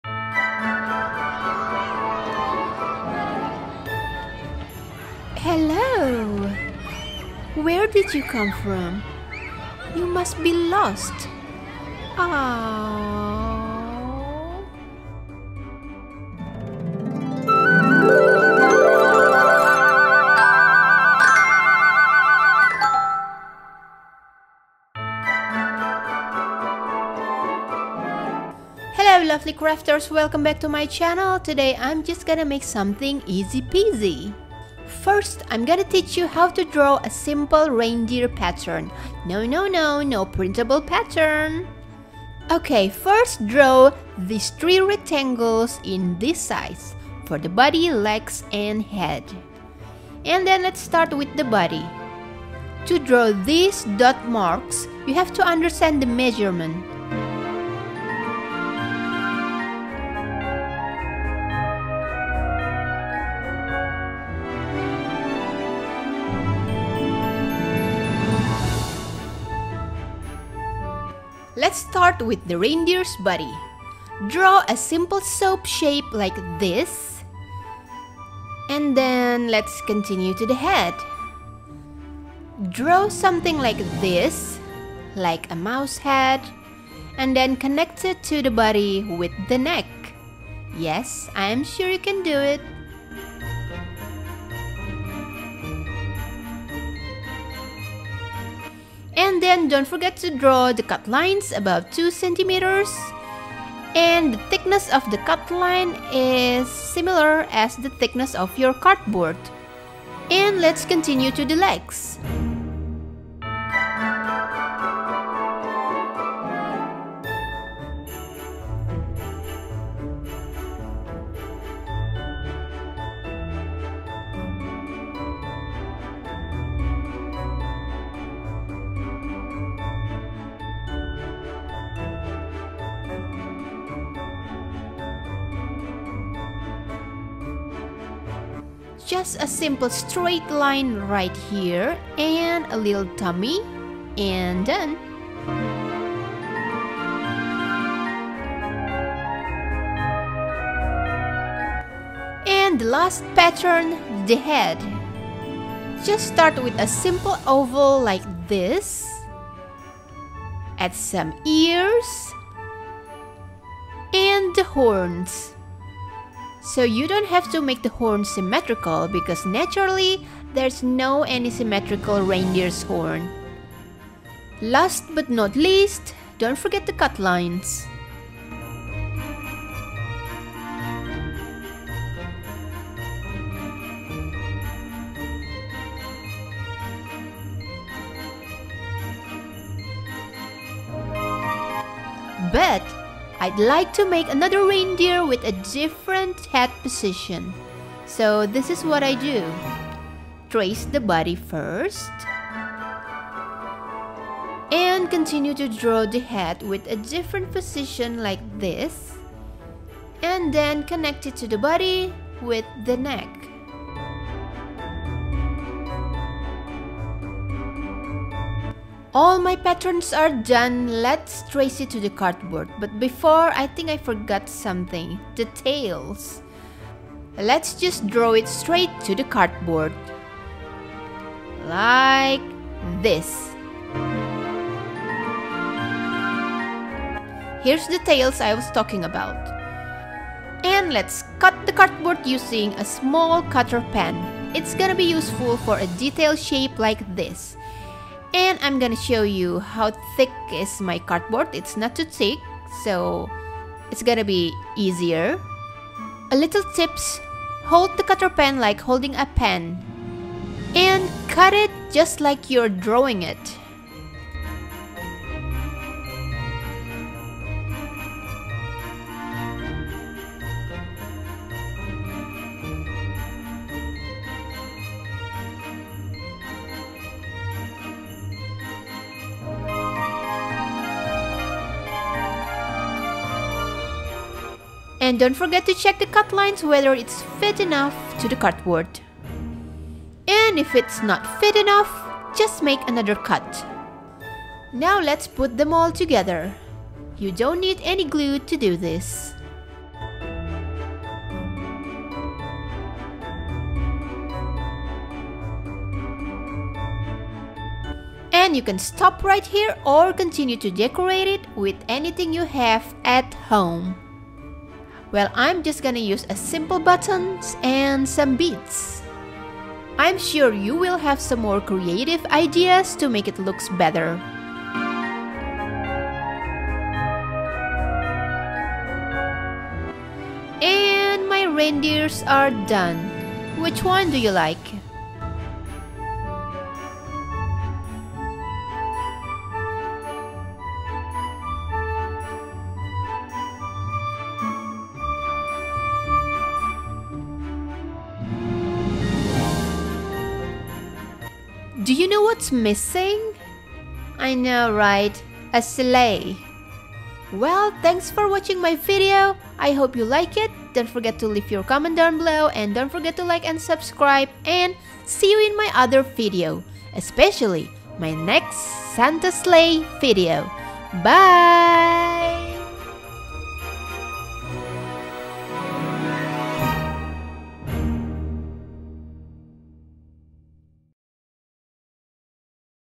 DIY Cardboard Reindeer
Let's make this easy cardboard reindeer as your Christmas decoration at home. Or you can make it bigger and better.